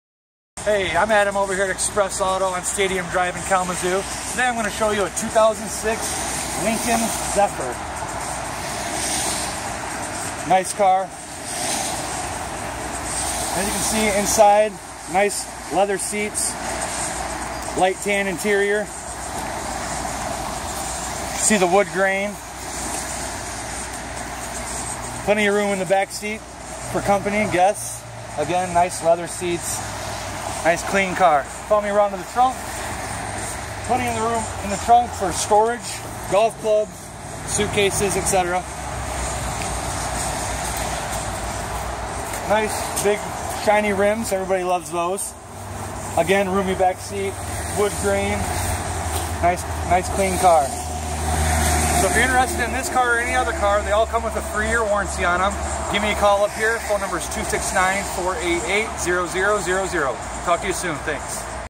2006 Lincoln Zephyr | Adam @ Stadium Drive Express Auto
2006 Lincoln Zephyr. A magnificent full-sized car. This is real luxury. Leather seats, wood grain interior, roomy back seat.